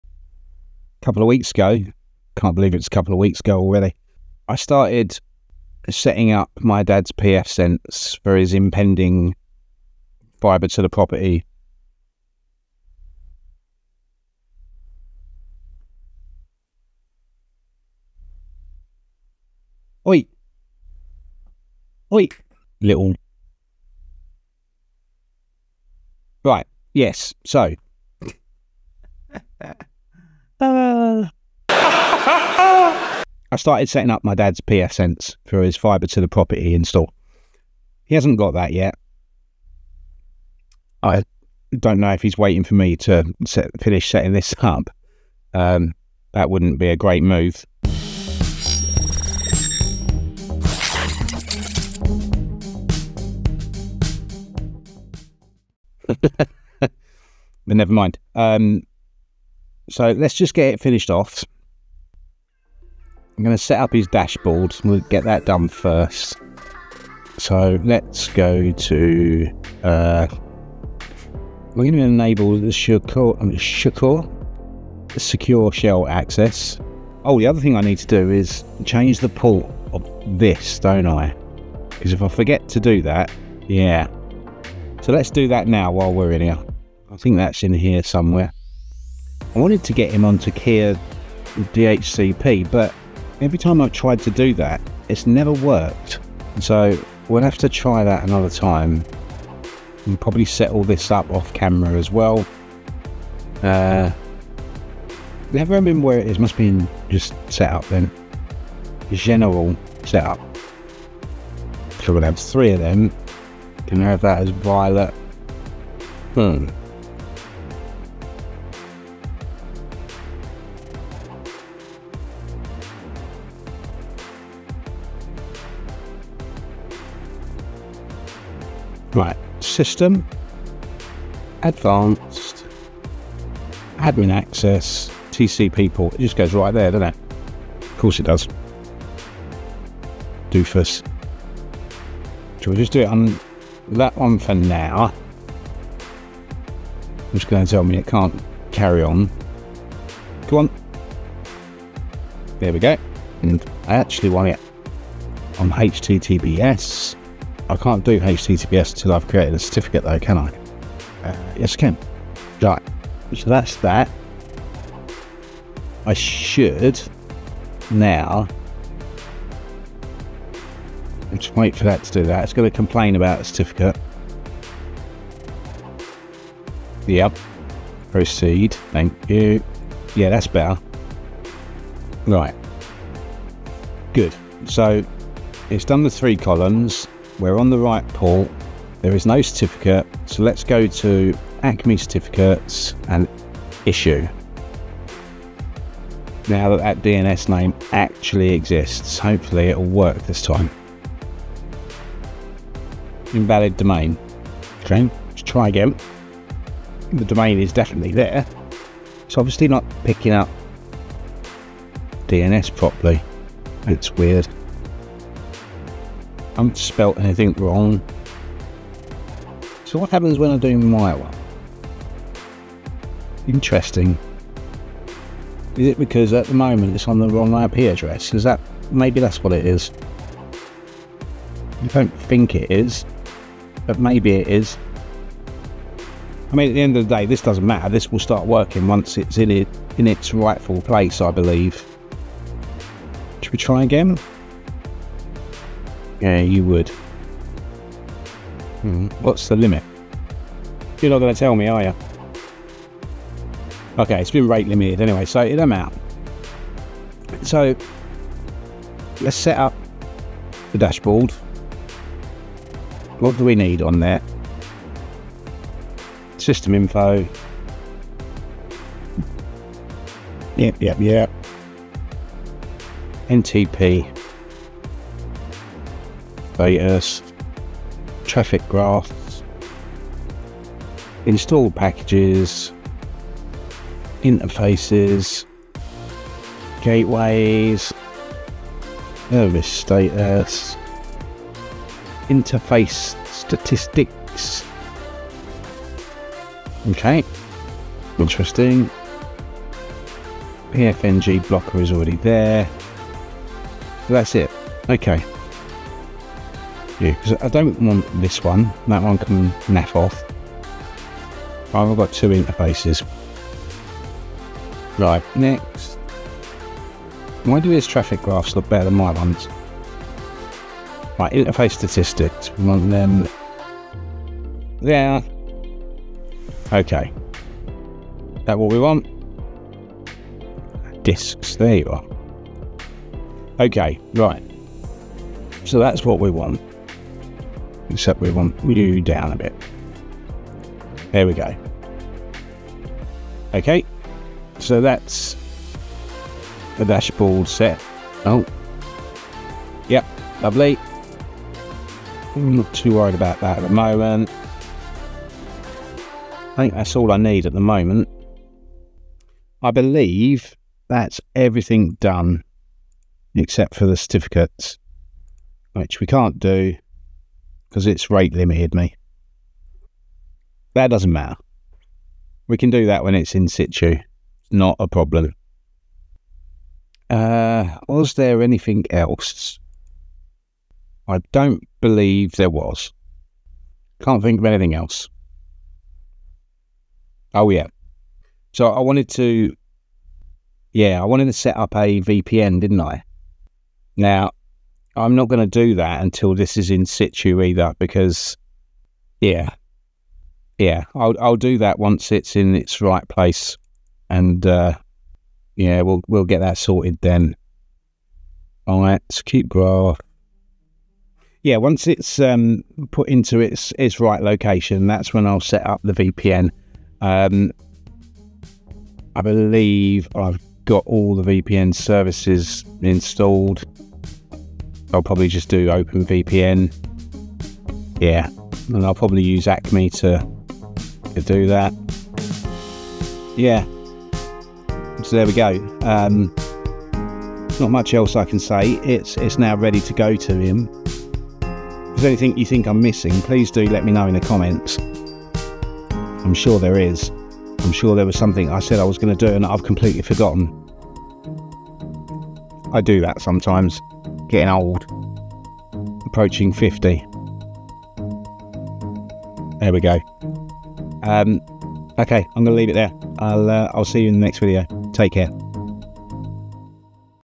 What Firewall should I set up for my Dad's Fiber to the Property? Part 2
Part 2 of this video. There are a lot of firewall/router choices out there, which one will I pick for a Fiber to the Property install for my Dad?

Don't forget to check out my Discord server where you can talk open source operating systems and software

FOSS
FreeBSD Handbook
FreeBSD Networking
FreeBSD PKG
FreeBSD ports tree